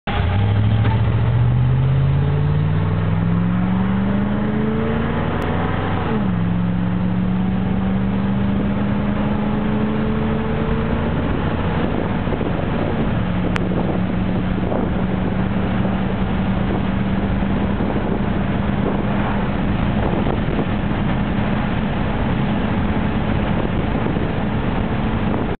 1978 F-150 0-60 attempt 1
I just thought I'd try to do a 0-60 run in my truck, I didn't get a good launch, and it would be alot faster with manual shifts. Trying to hold the camera, and drive an old truck, while paying attention to the road is a little tricky.

It has the 351M with C6 auto. Its 4x4 and it has 31" tires so 55 on the speedo is actually 60. Oh, did I mention its got 120,000 miles on it?